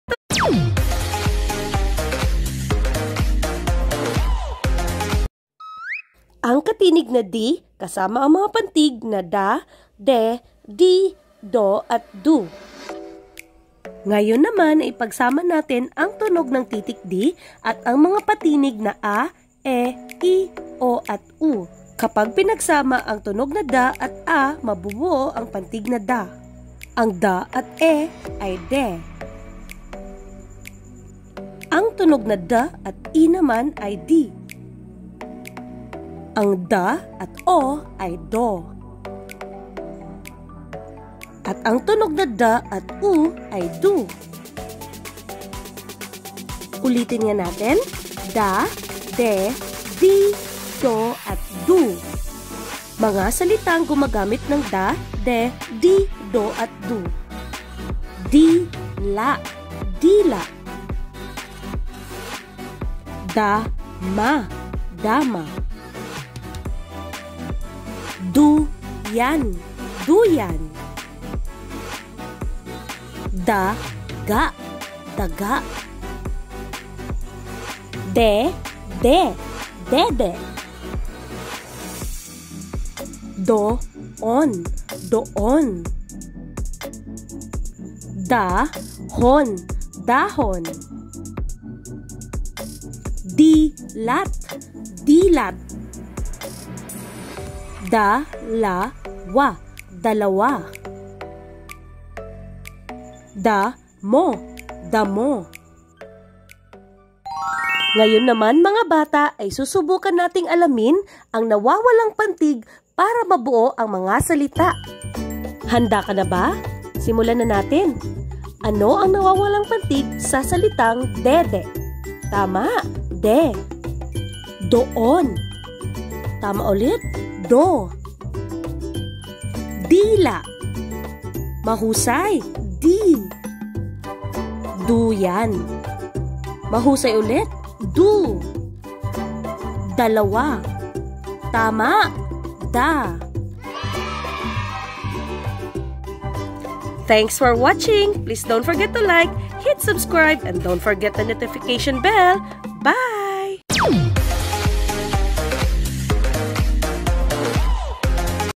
Ang katinig na titik Dd kasama ang pantig na da, de, di, do at du (Salitang may da, de, di, do, du)
(PRESCHOOLERS)
Mga larawang nagsisimula sa titik Dd